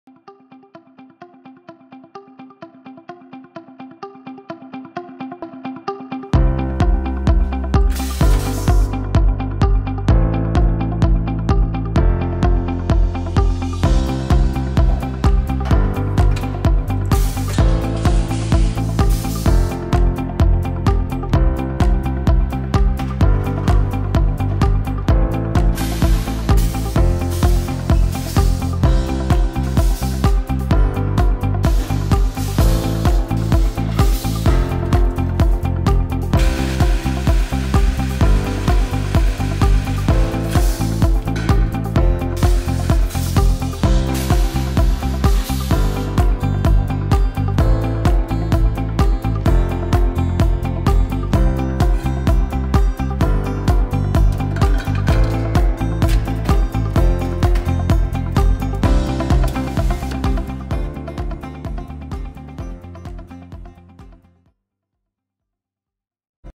High Volume Semi-Automated Can Opener.
Edlund's High Volume Semi-Automated Can Opener.   Opens, de-lids and empties up to 15 cans per minute, with a single operator.